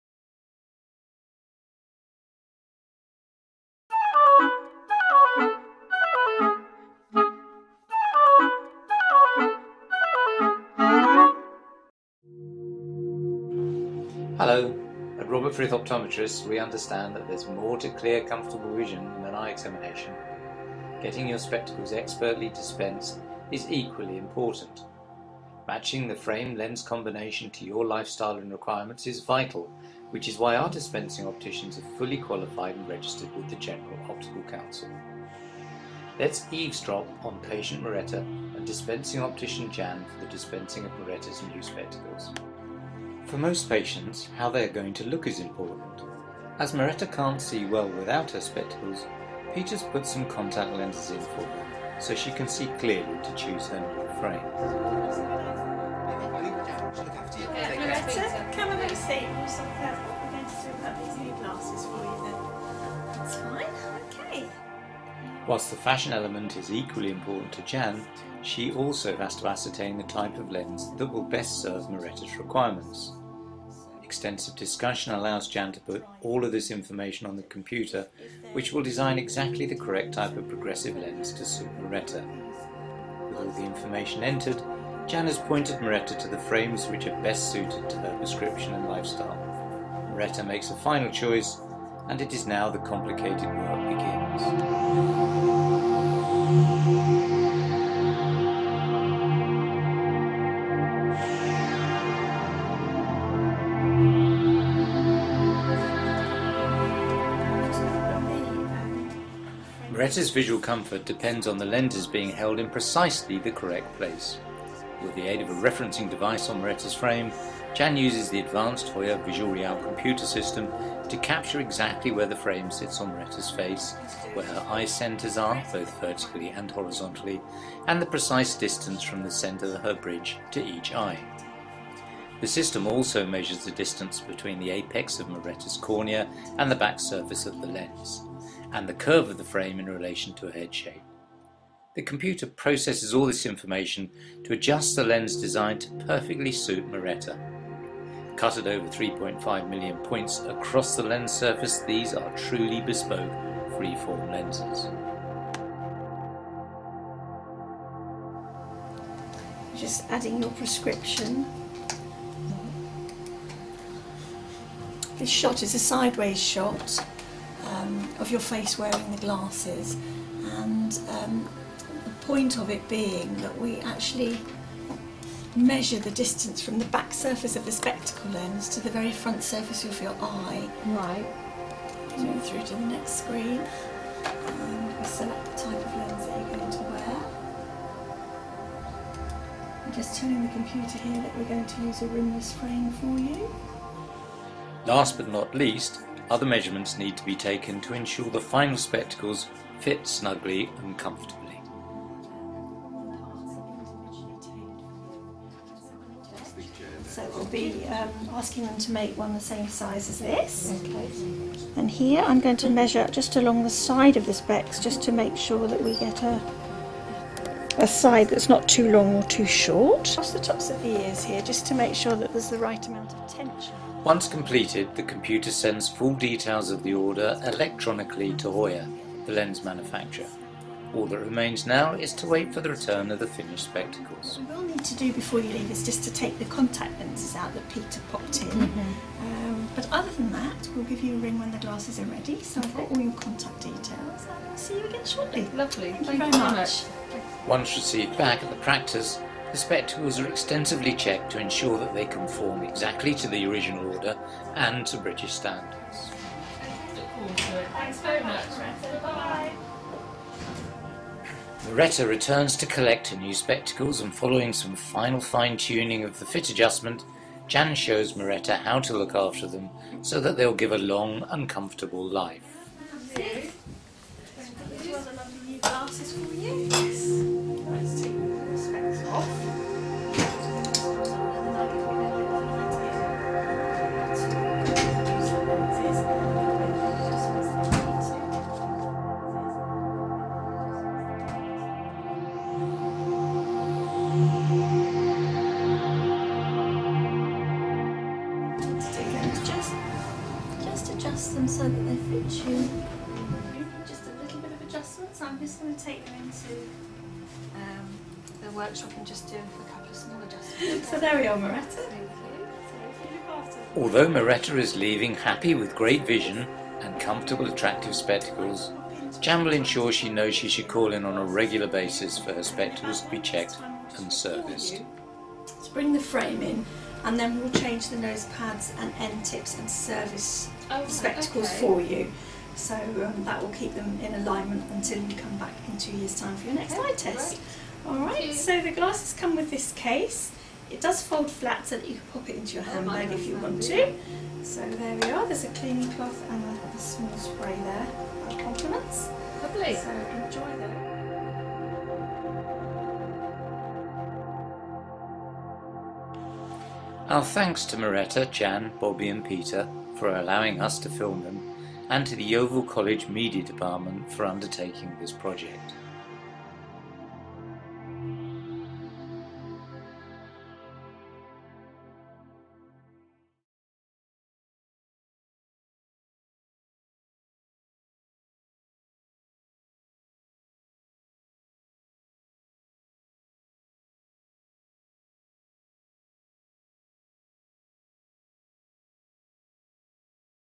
Getting new glasses
There is more to buying a new pair of spectacles than choosing a frame. With the help of the staff and patients at Blandford David Goad from Robert Frith Optometrists takes us the the process of having a new pair of spectacles properley dispensed.